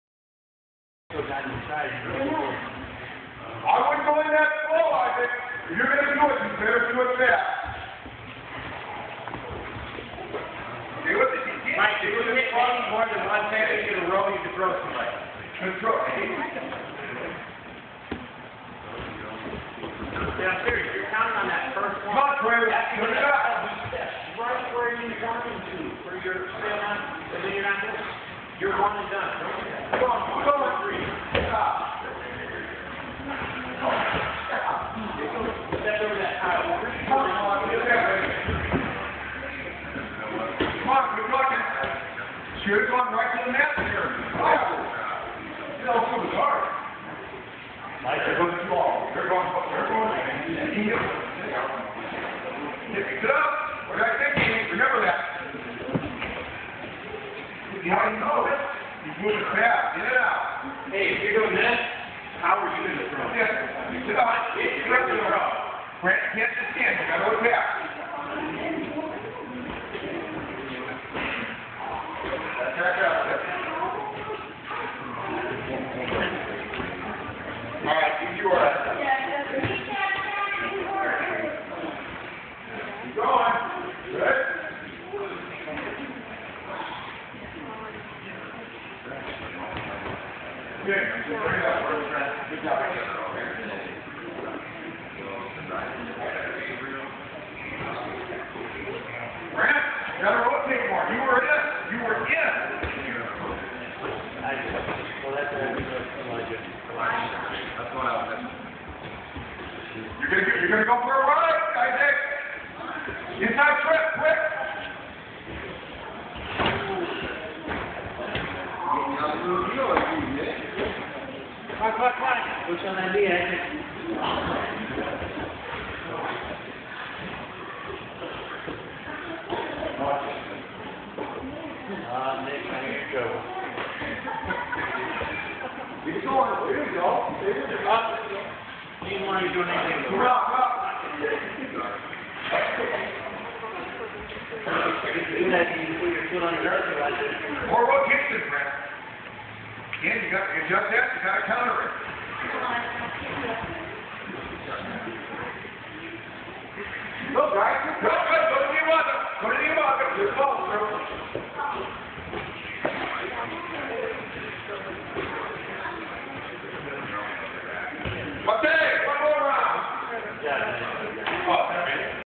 Danny Gillette Judo practice 7 years olds.
I am not sure which throws they are making. I think, both like Ippon-seoi-nage but end result is a reap of some kind or Tia-o-toshi. They are a good match for Judo practice. Danny Gillette Starts on left of screen.